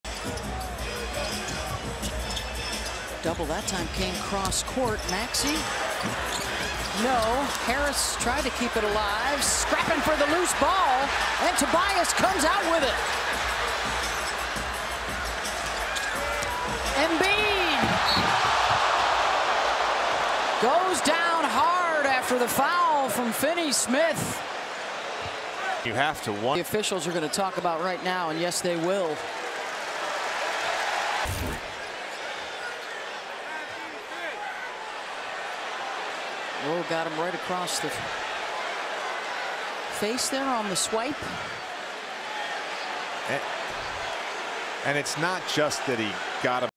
JOEL EMBIID PRETENDED TO PASS OUT! FINED 100,000 FOR FLOPPING & ACTING SKILLS! LOL! HAHA!
From Curry to FIGHTING WITH KLAY on the bench to LEBRON getting in Laker Teammate's face during a timeout! NBA hidden chats!